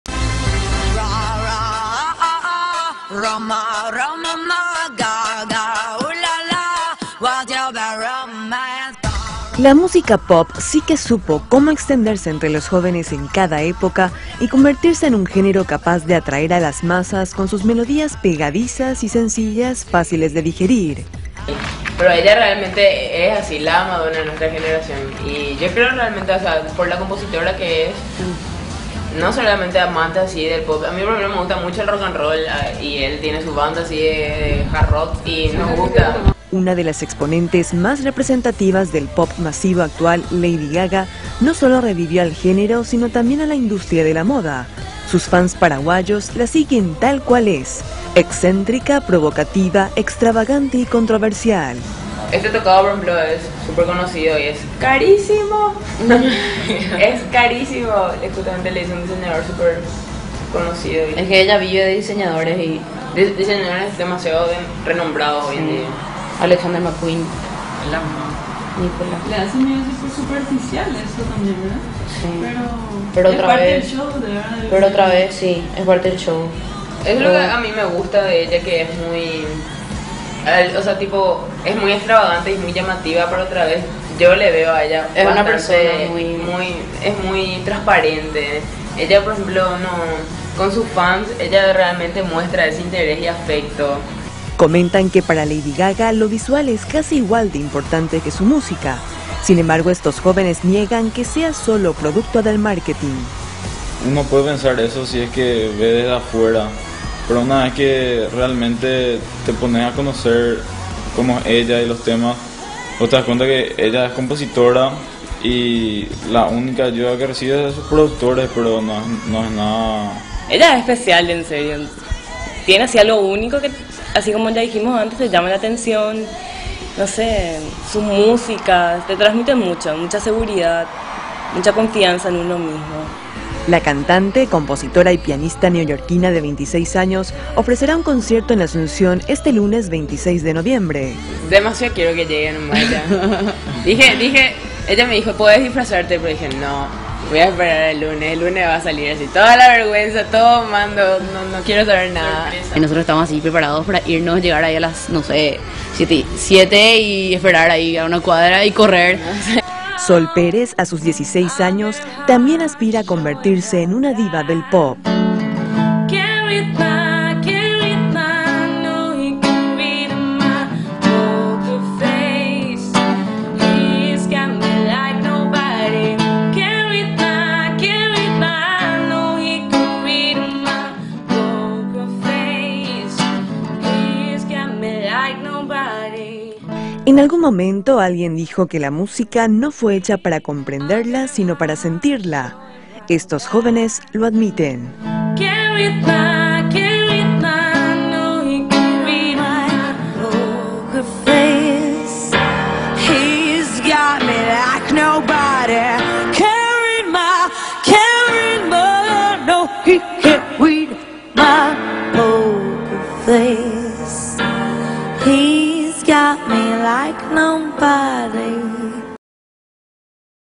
Fanáticos aguardan el concierto de Lady Gaga en Py
La Gaga Manía se percibe cada vez más especialmente entre la gente joven ante su presentación en vivo por primera vez en Paraguay. Estuvimos con un grupo de fanáticos de la excéntrica cantante norteamericana a quien califican como la Madonna de la nueva generación teen. Emitido por Telefuturo. Excelente edición de Manuel Garay.